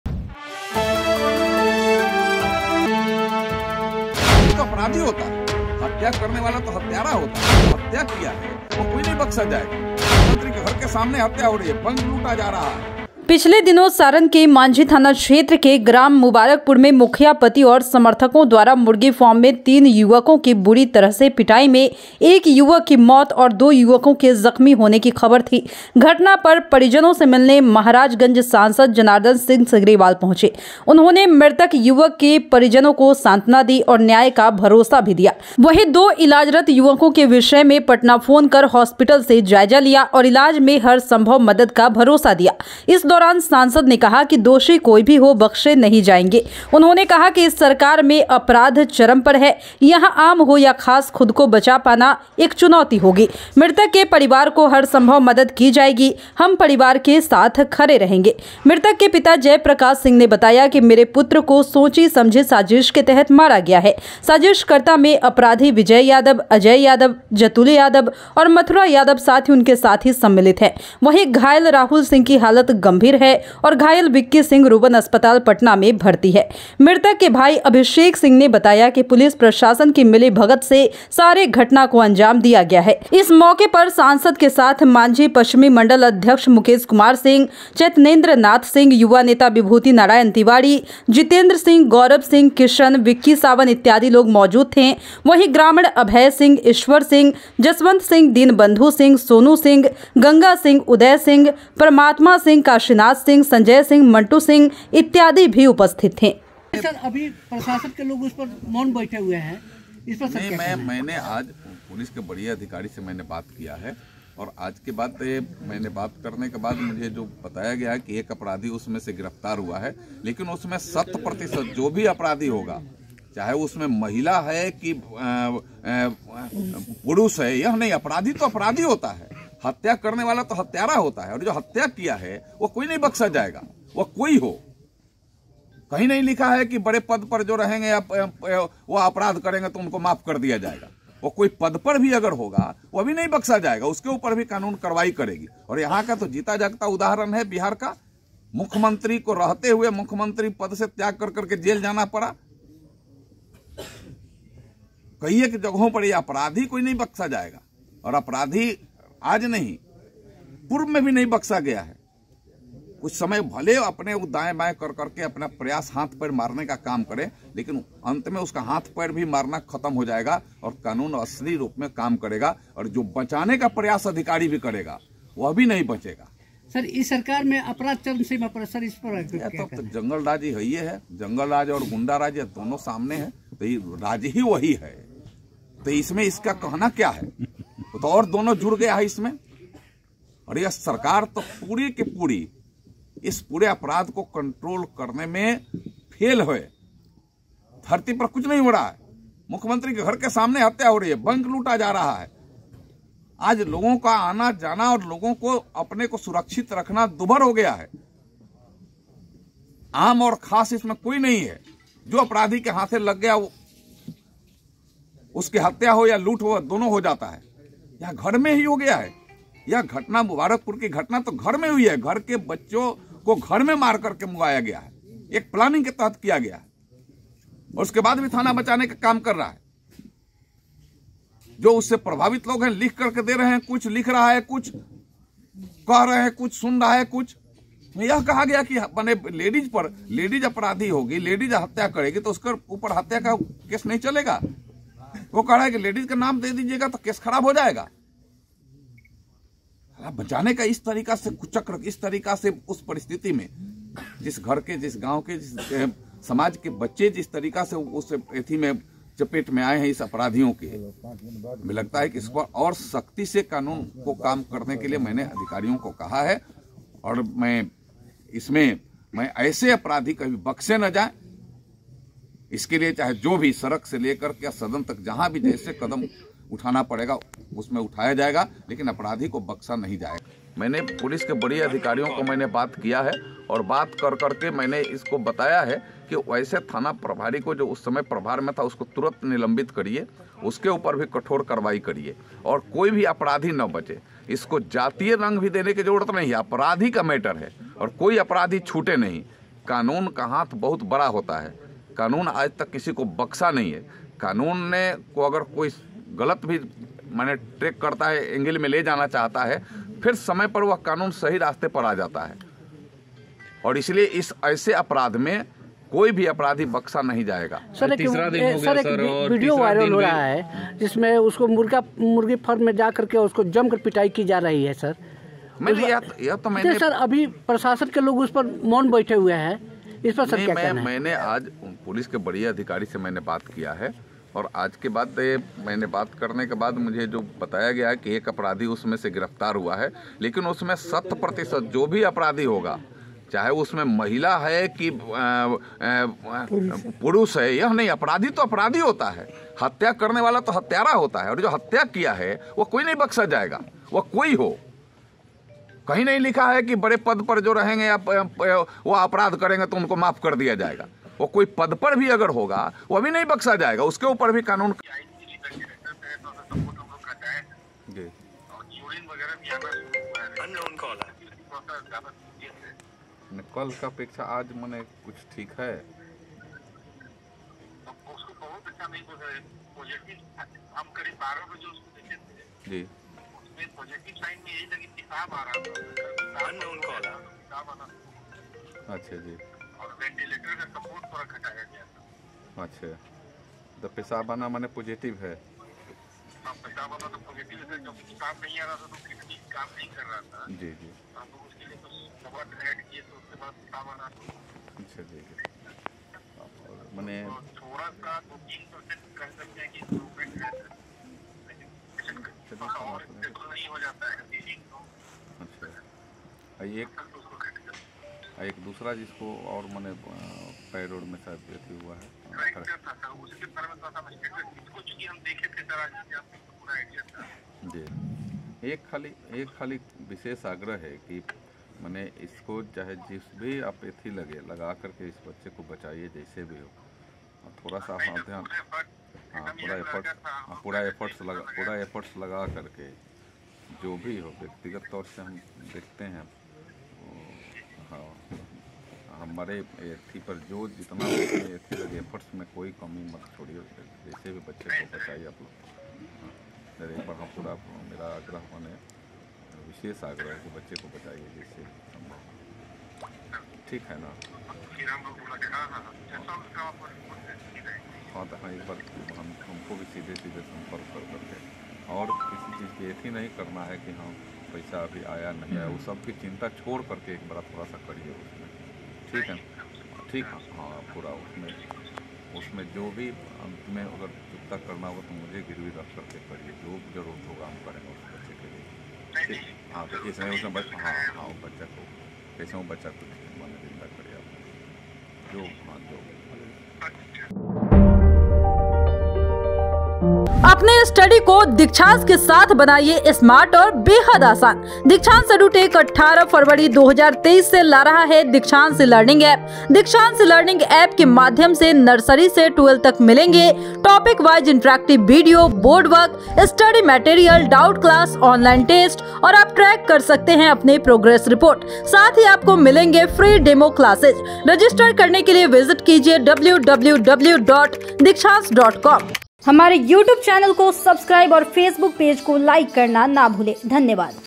सारण के मांझी कांड का जायजा लेने पहुंचे सांसद सिग्रीवाल ग्रामीणों और परिजनों को दिलाया न्याय..
सारण के मांझी कांड का जायजा लेने पहुंचे सांसद सिग्रीवाल ग्रामीणों और परिजनों को दिलाया न्याय का भरोसा सुनिए |